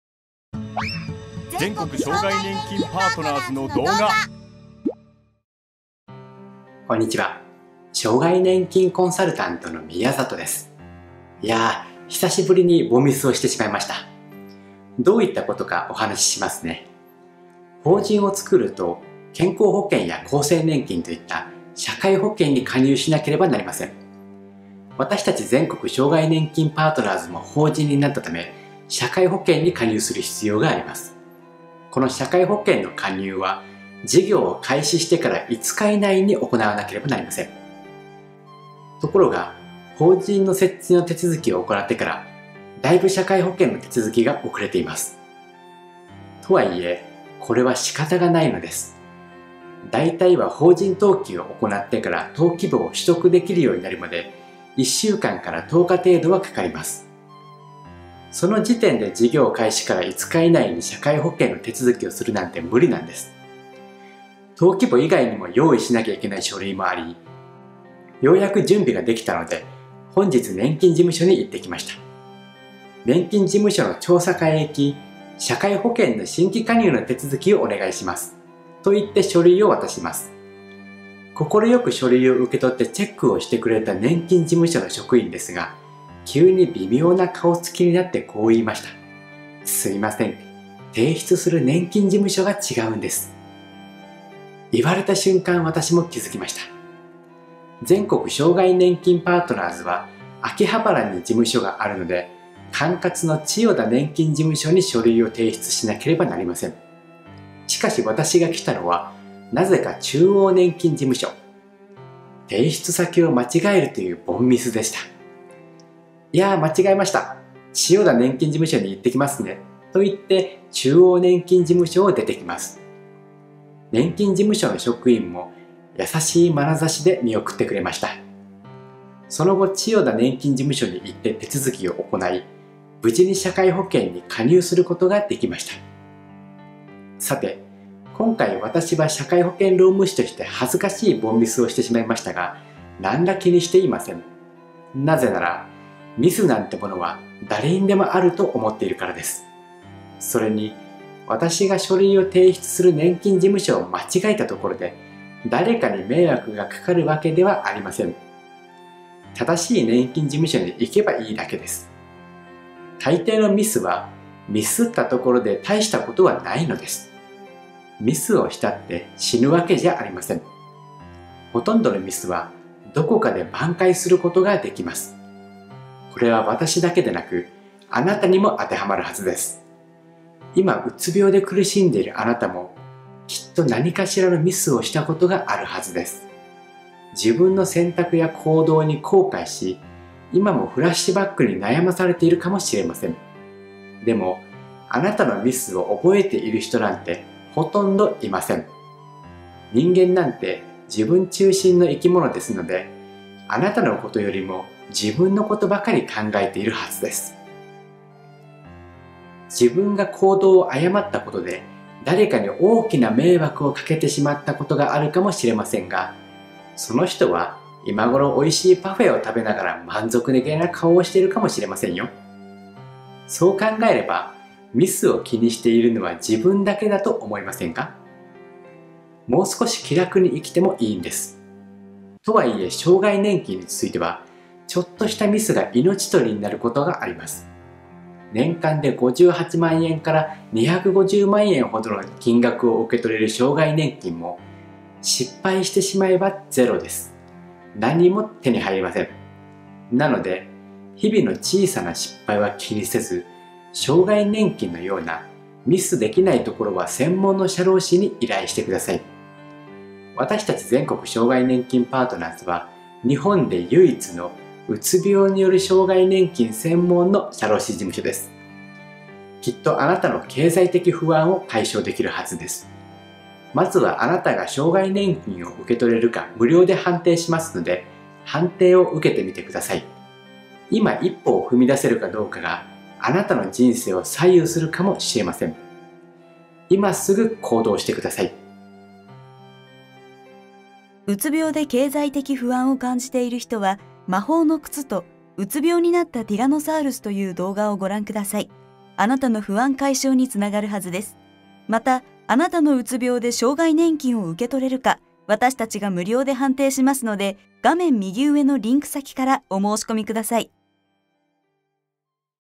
ミスなんてどうにでもなる！【社会保険労務士法人全国障害年金パートナーズ】
生きていればたくさんのミスをしますが、意外とどうにかなるものです。
【社会保険労務士法人全国障害年金パートナーズ】
障害年金の無料判定はこちら